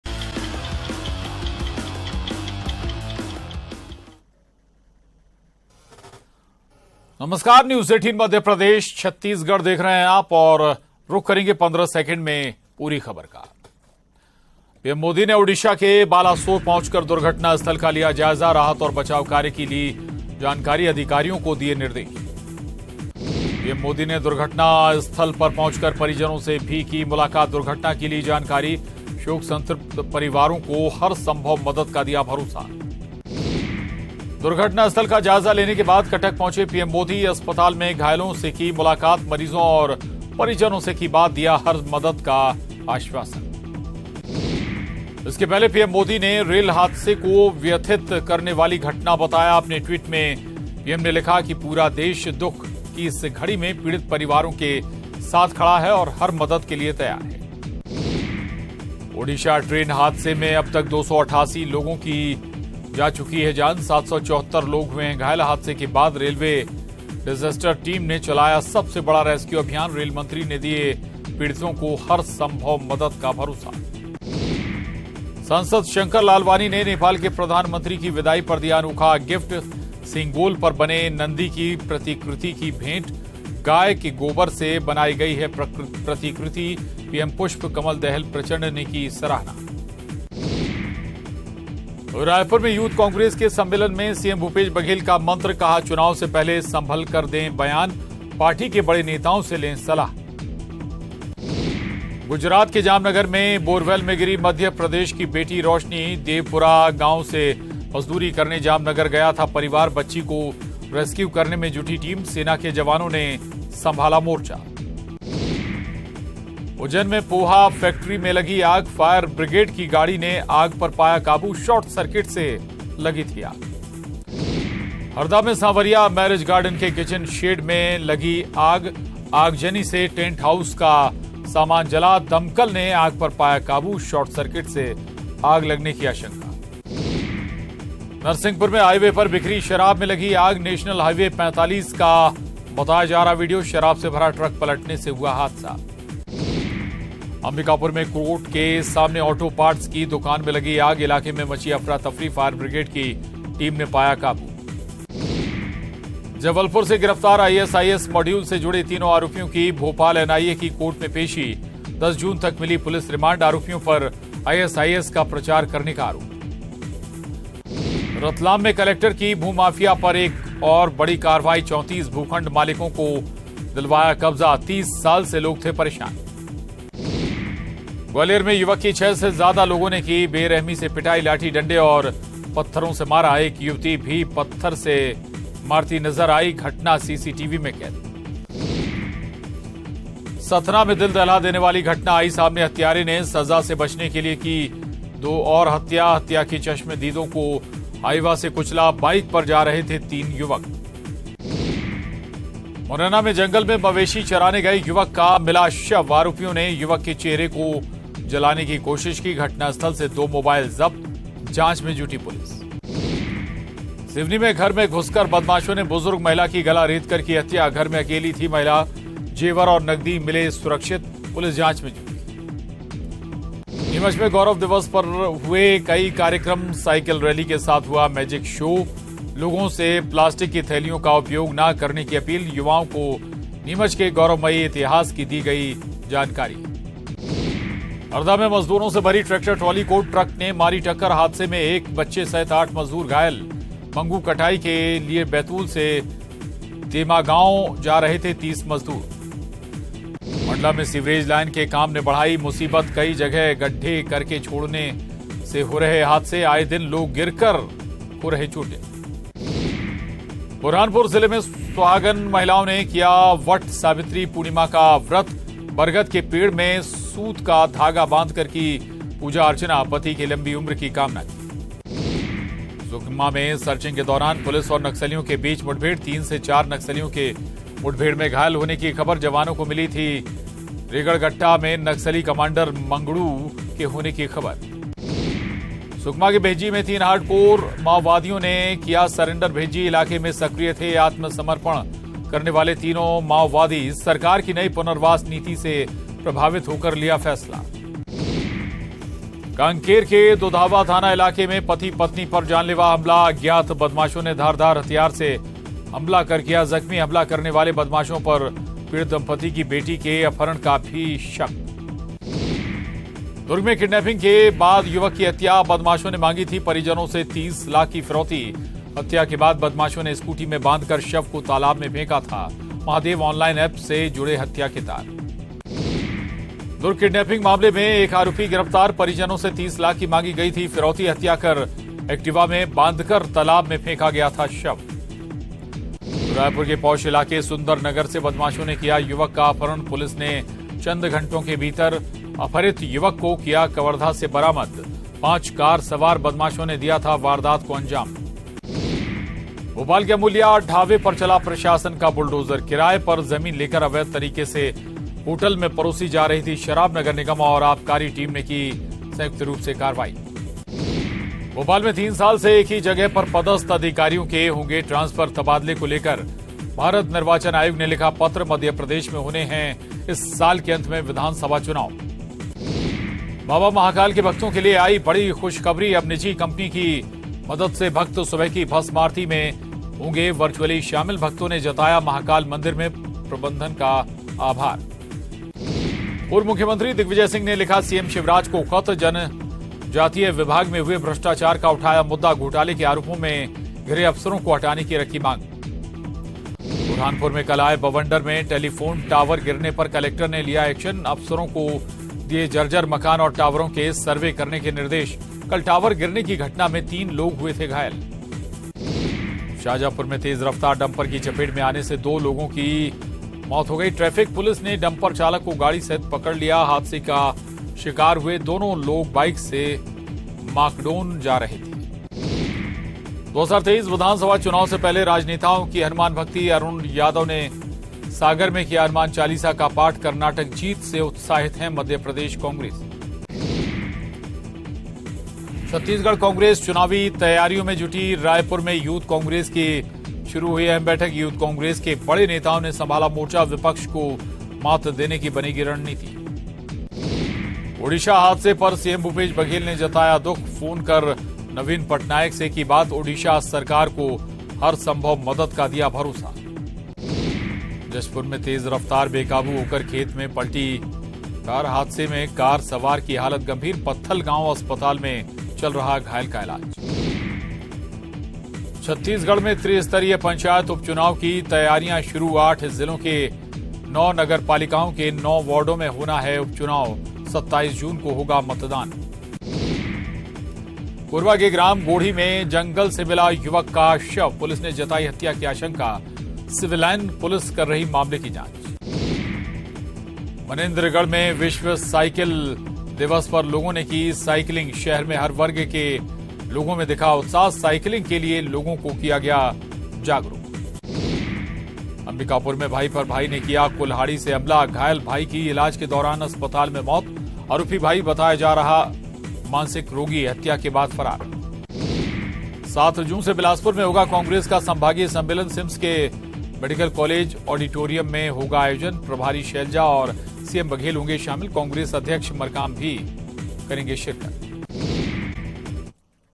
Top Headlines : गांव से शहर तक हर ख़बर पर पैनी नज़र | देखिए '15 सेकेंड में ख़बर' | MP CG Top News
00:00 Intro
00:16 PM Modi पहुंचे घटनास्थल
01:32 पीएम मोदी ने की मुलाकात